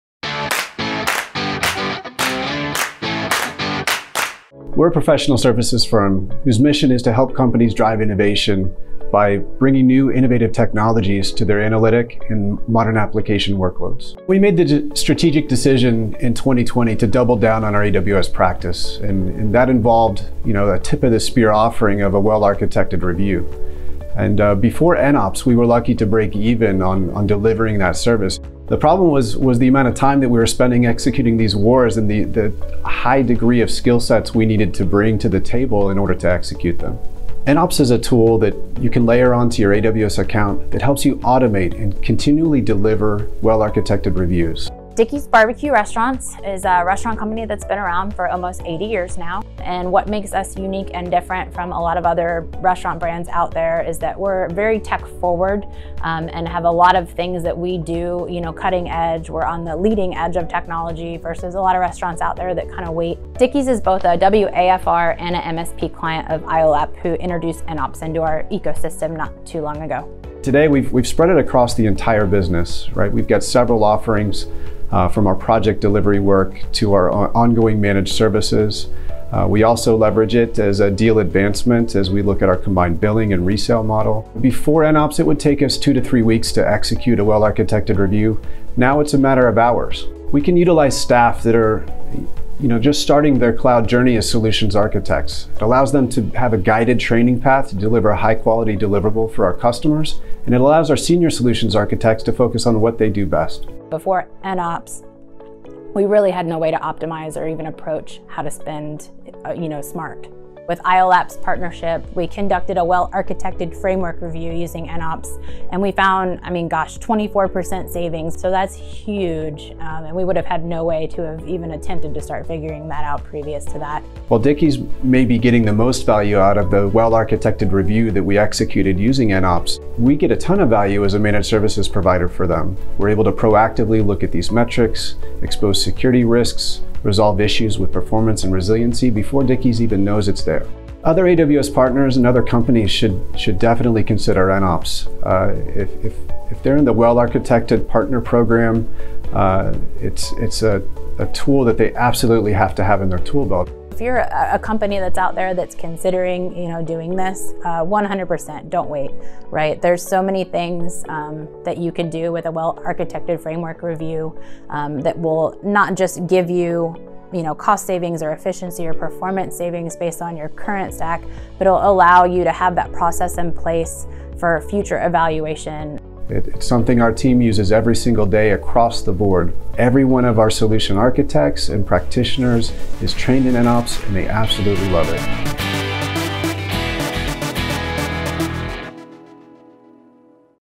iOLAP & Dickey’s Barbecue Pit use nOps to get and stay Well-Architected.
Watch our new three-minute video to learn how Dickey’s Barbecue Pit benefitted from an AWS Well-Architected Framework Review (WAFR) conducted by iOLAP, an nOps Partner. In business for nearly 80 years, Dickey’s prides itself on being a tech-forward restaurant brand.

On an ongoing basis, iOLAP uses nOps to monitor Dickey’s infrastructure health for cost optimization, security, reliability, performance efficiency, and operational excellence. As a result, iOLAP can proactively resolve issues, often before Dickey's is aware of them.